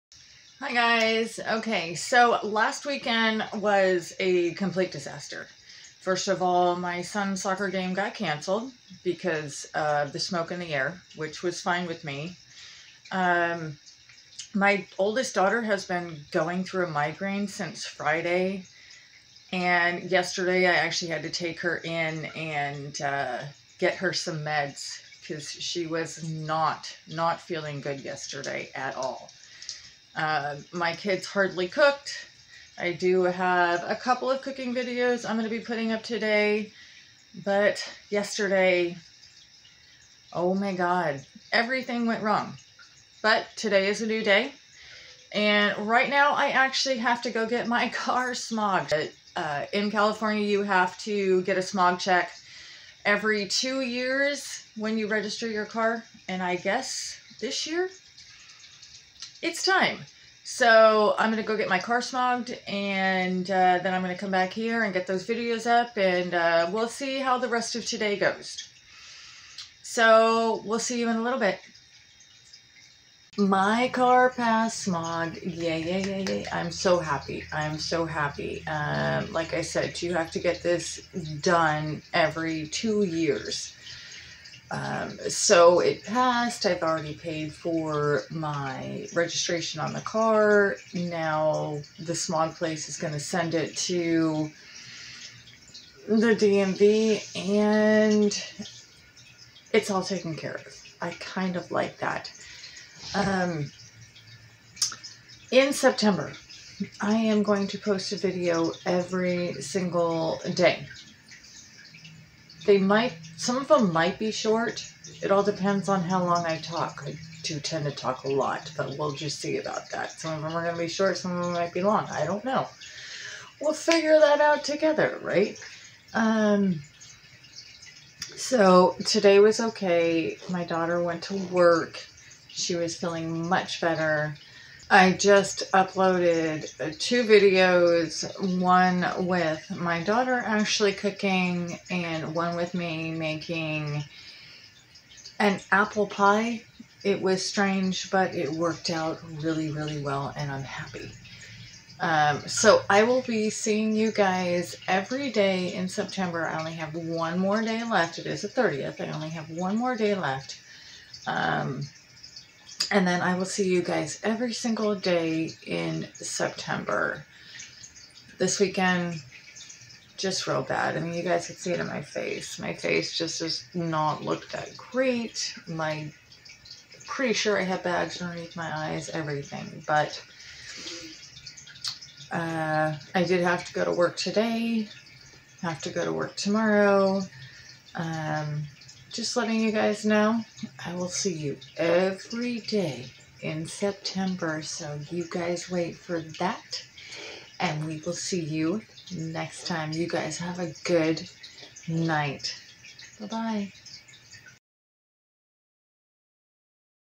MY CAR PASSED SMOG
What a crazy weekend and had to get my car registered.